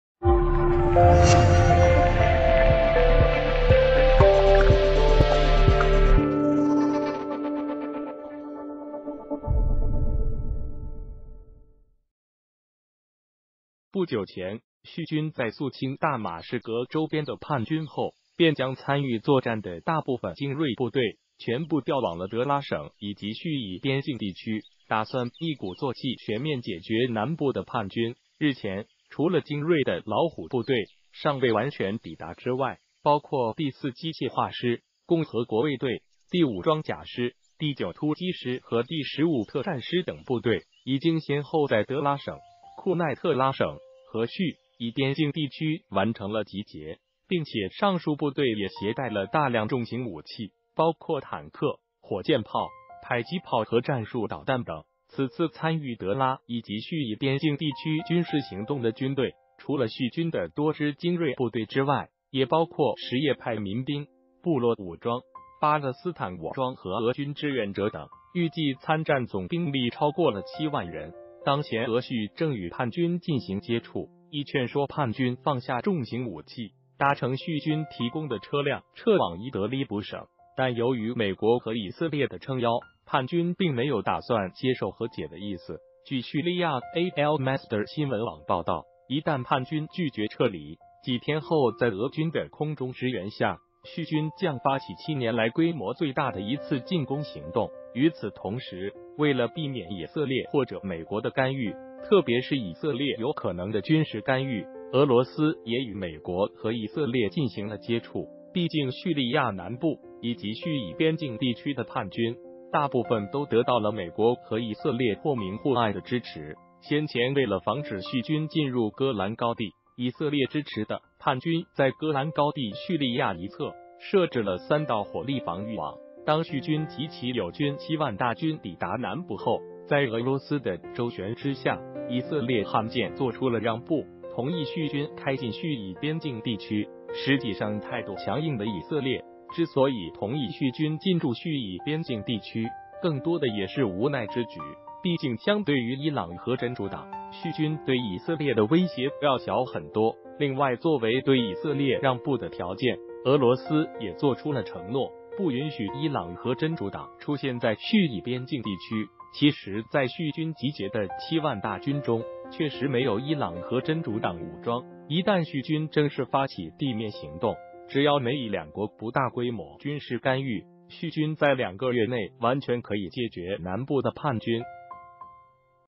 俄罗斯出面周旋，以色列终于让步，7万叙军即将发起最大进攻
Many thanks.不久前叙军在肃清大马士革周边的叛军后，便将参与作战的大部分精锐部队，全部调往了德拉省以及叙以边境地区，打算一鼓作气全面解决南部的叛军。日前，除了精锐的老虎部队尚未完全抵达之外，包括第四机械化师、共和国卫队、第五装甲师、第九突击师和第15特战师等部队，已经先后在德拉省、库奈特拉省和叙以边境地区完成了集结，并且上述部队也携带了大量重型武器，包括坦克、火箭炮、迫击炮和战术导弹等。此次参与德拉以及叙以边境地区军事行动的军队，除了叙军的多支精锐部队之外，也包括什叶派民兵、部落武装、巴勒斯坦武装和俄军“志愿者”等，预计参战总兵力超过了7万人。当前俄叙正与叛军进行接触，以劝说叛军放下重型武器，搭乘叙军提供的车辆撤往伊德利卜省，但由于美国和以色列的撑腰，叛军并没有打算接受和解的意思。据叙利亚Al-Masdar新闻网报道，一旦叛军拒绝撤离，几天后在俄军的空中支援下，叙军将发起7年来规模最大的一次进攻行动。与此同时，为了避免以色列或者美国的干预，特别是以色列有可能的军事干预，俄罗斯也与美国和以色列进行了接触，毕竟叙利亚南部以及叙以边境地区的叛军，大部分都得到了美国和以色列或明或暗的支持。先前为了防止叙军进入戈兰高地，以色列支持的叛军在戈兰高地叙利亚一侧，设置了三道火力防御网。当叙军及其友军7万大军抵达南部后，在俄罗斯的周旋之下，以色列罕见做出了让步，同意叙军开进叙以边境地区。实际上态度强硬的以色列，之所以同意叙军进驻叙以边境地区，更多的也是无奈之举，毕竟相对于伊朗和真主党，叙军对以色列的威胁要小很多。另外作为对以色列让步的条件，俄罗斯也做出了承诺，不允许伊朗和真主党出现在叙以边境地区，其实在叙军集结的7万大军中，确实没有伊朗和真主党武装。一旦叙军正式发起地面行动，只要美以两国不大规模军事干预，叙军在两个月内完全可以解决南部的叛军。